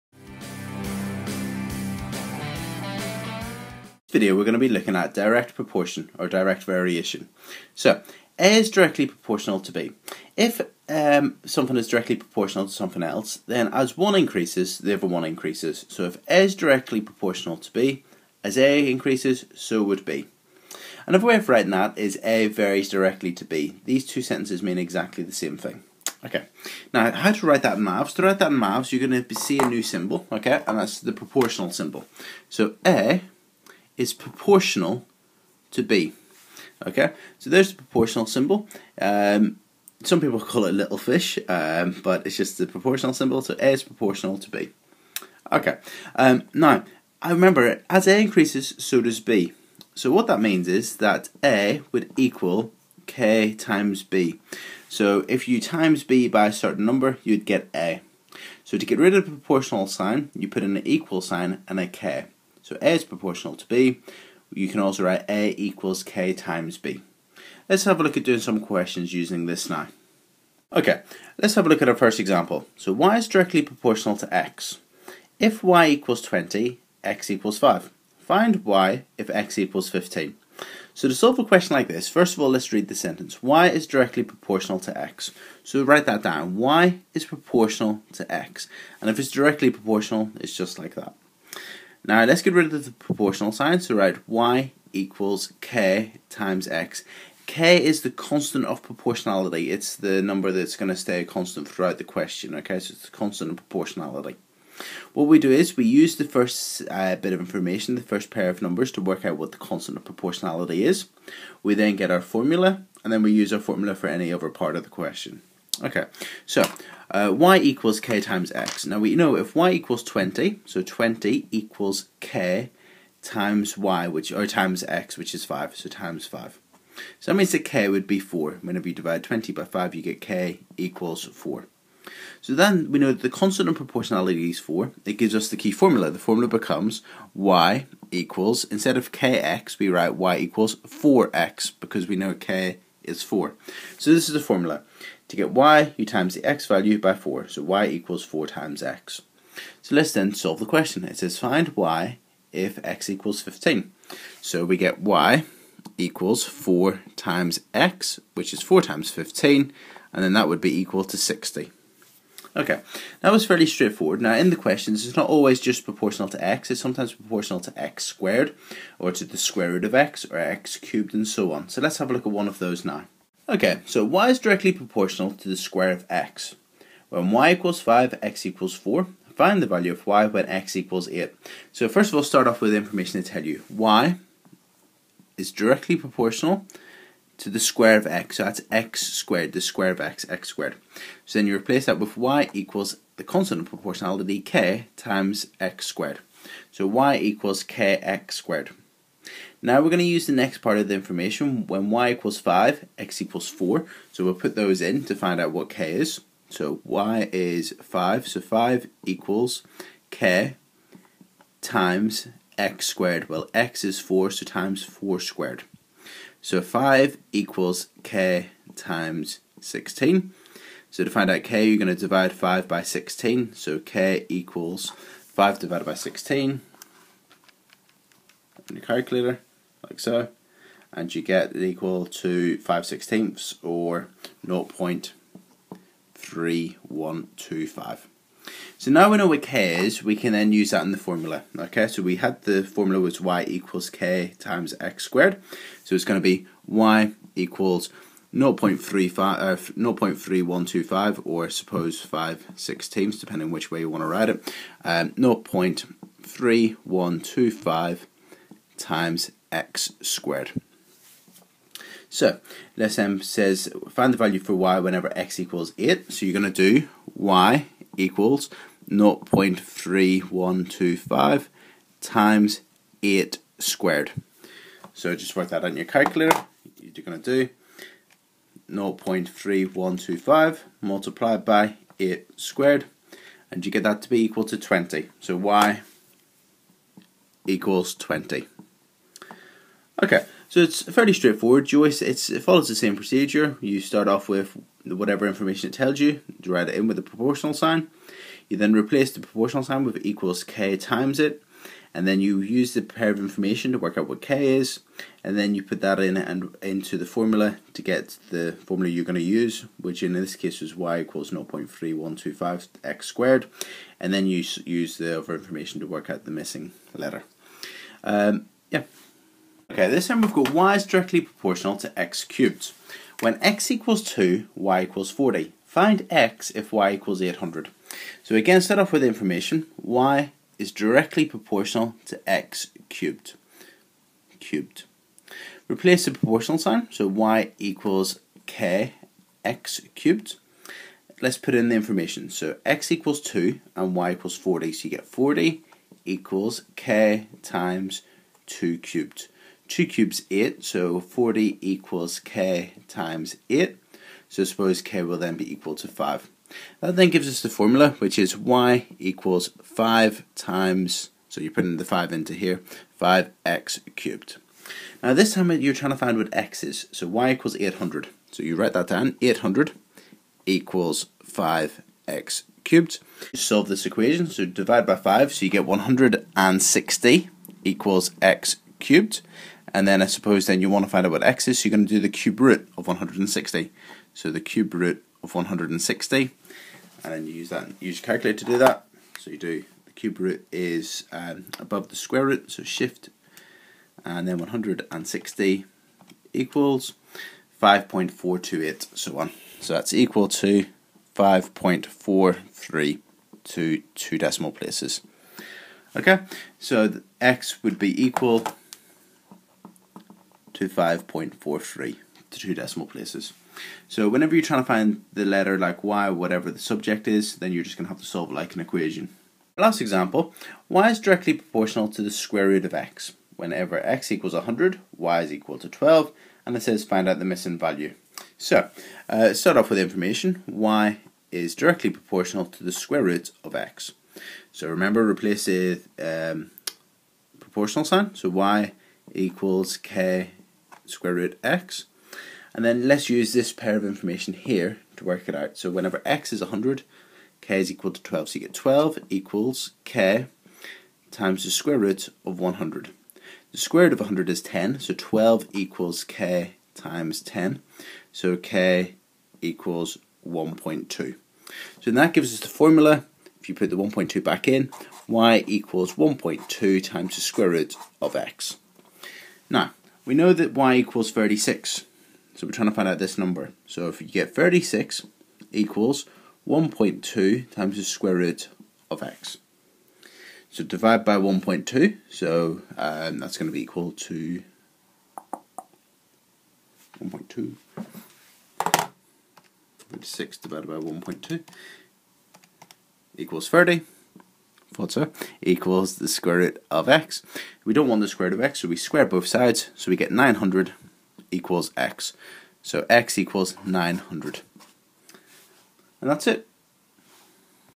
Direct Proportion - Corbettmaths
Corbettmaths - This video explains how to solve direct proportion questions. It introduces the proportionality symbol and the constant of proportionality.